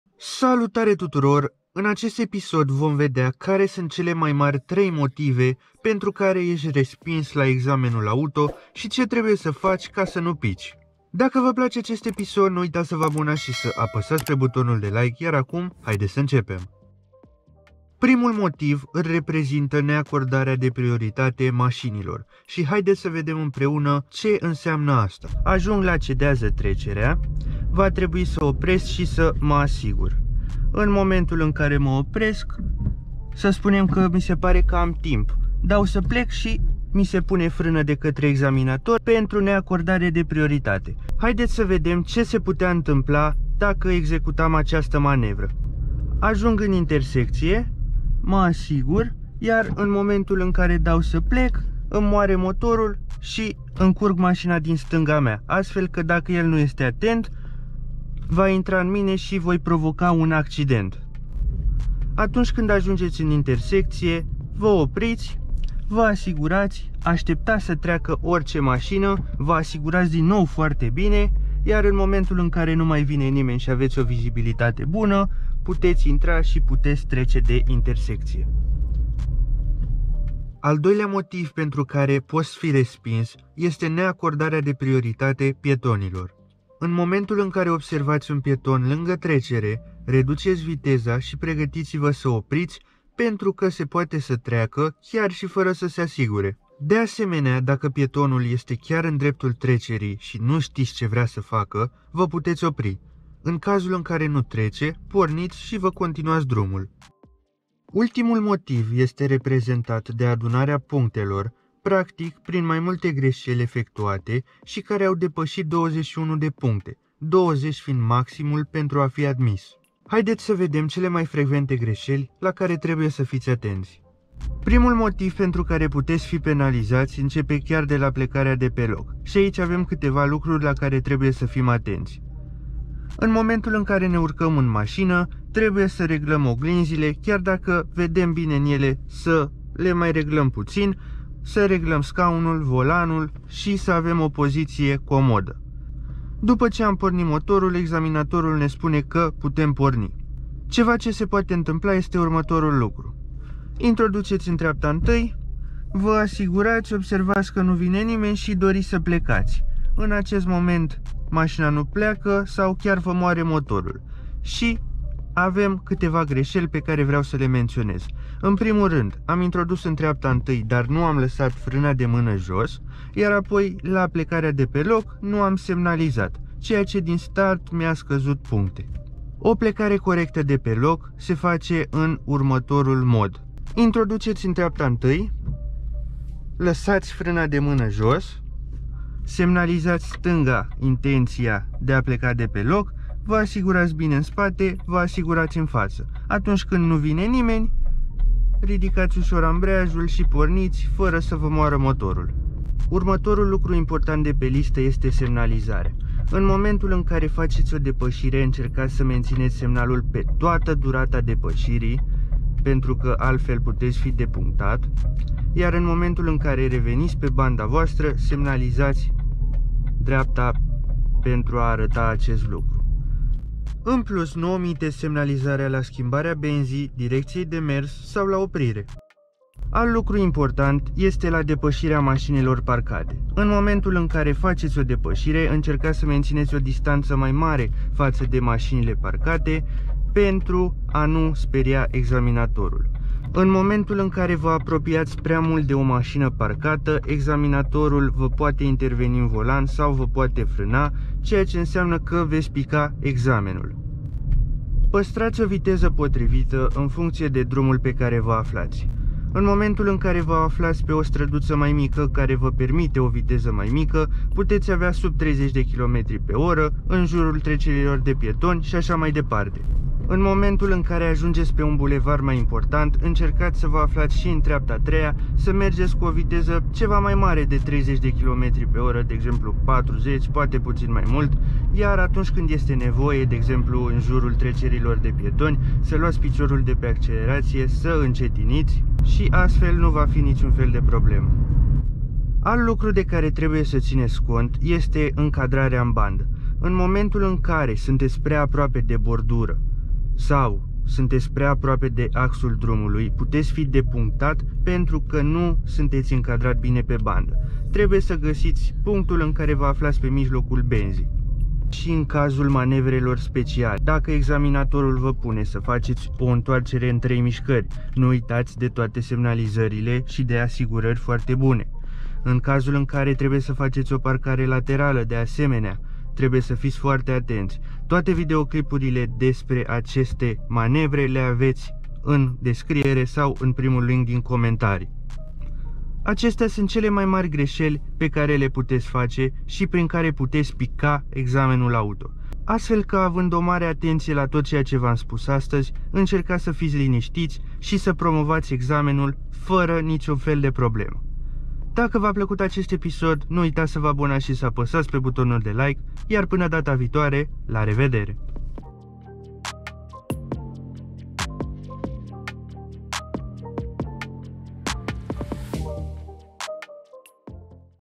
3 MOTIVE pentru care ESTI RESPINS la EXAMENUL AUTO
Astazi va arat  principalele motive pentru care sunteti respinsi la proba practica de condus.

Daca v-a placut, nu ezitati sa apasati pe butonul de like, sa va abonati si sa dati un share.
Camera CarVlog: GoPro HERO 8

SFATURI PENTRU TRASEU SI SALA